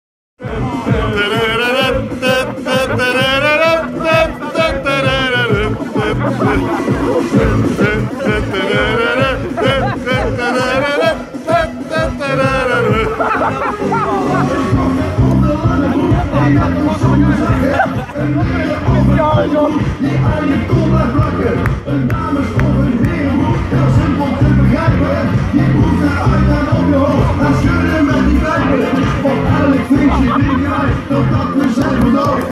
Broek uit op je hoofd
Mafkees trekt broek uit en zet hem op zijn hoofd bij de carnavalsoptocht CV de Damzwabbers in Zwabberdam  (Leidschendam )